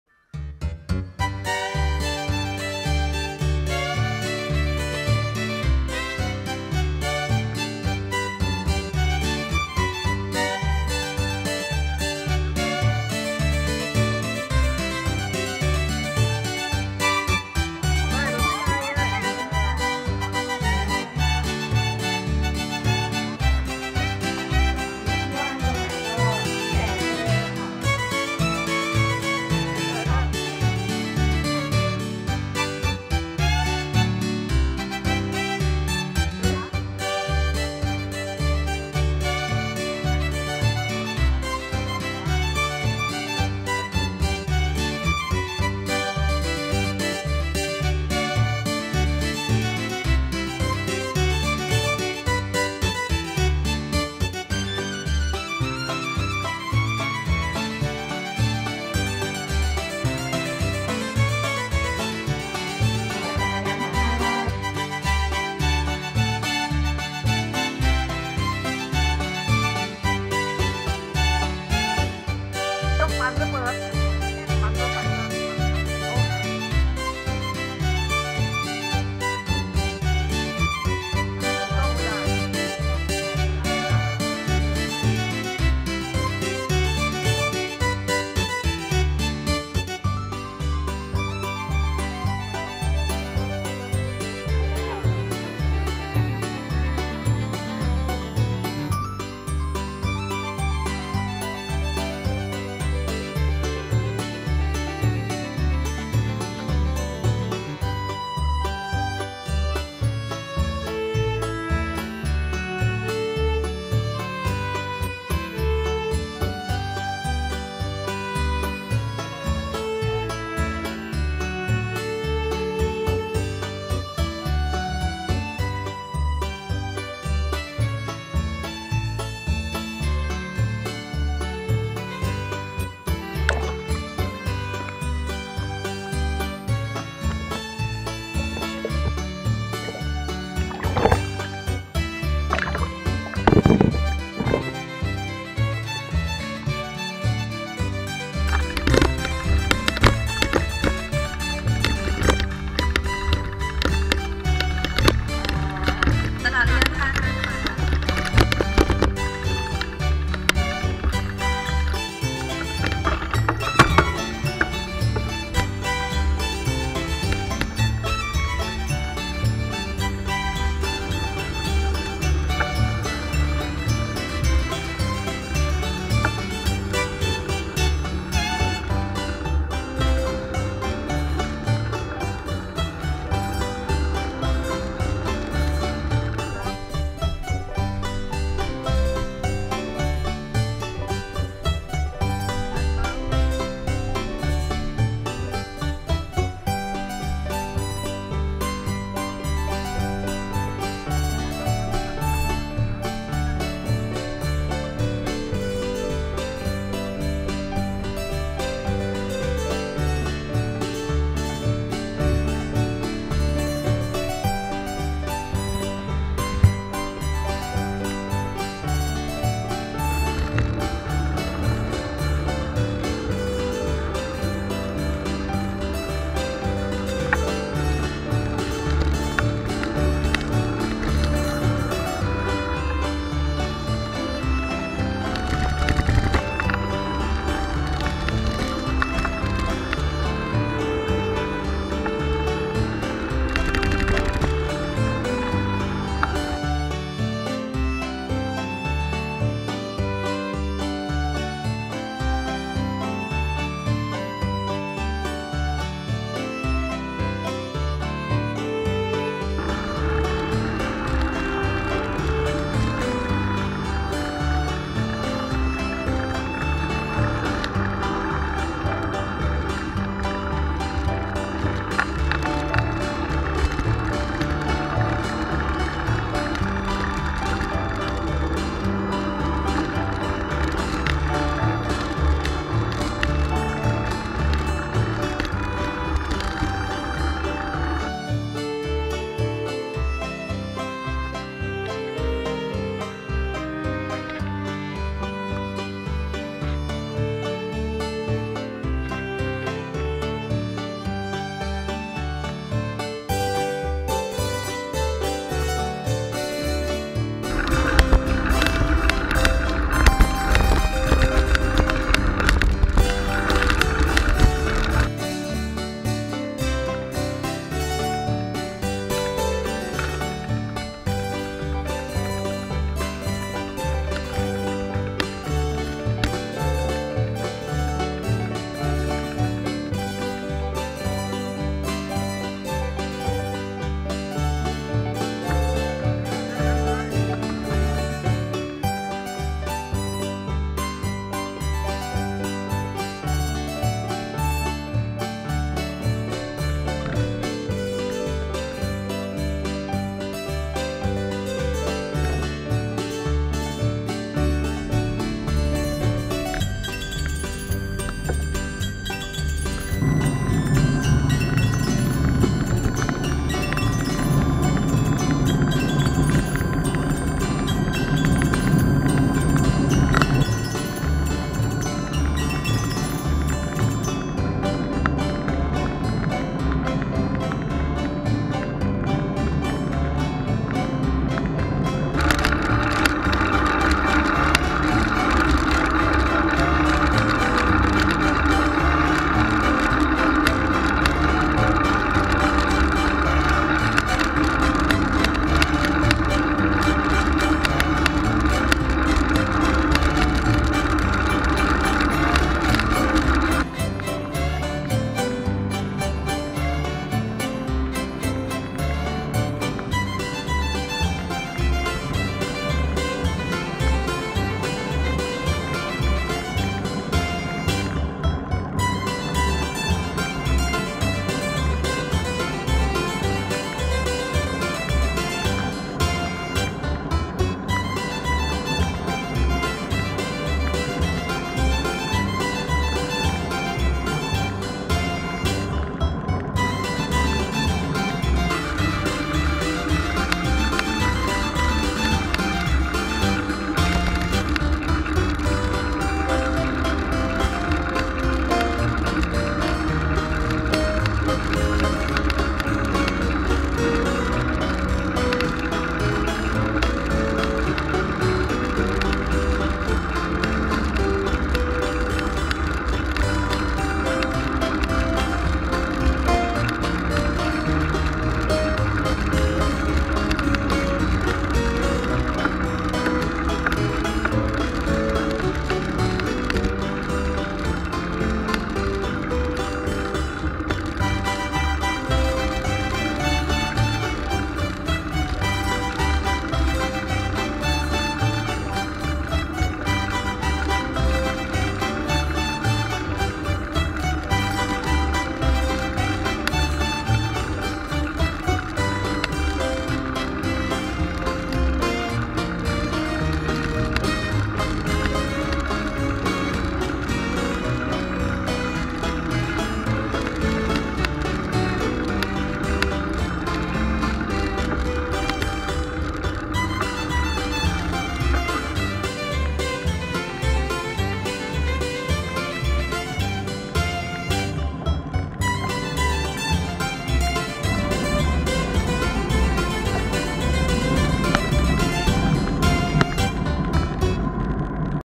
ปั่น ปั่น ปั่น เที่ยวทะเลเกาะเปริด
ปั่น ปั่น ปั่น ร่วมกันปล่อยกุ้งคืนสู่ธรรมชาติ  ปั่เที่ยวชมทะเลเกาะเปริด เลาะไปสู่โรงเรียน ตชด. บ้านน้ำแดง ต.บางชัน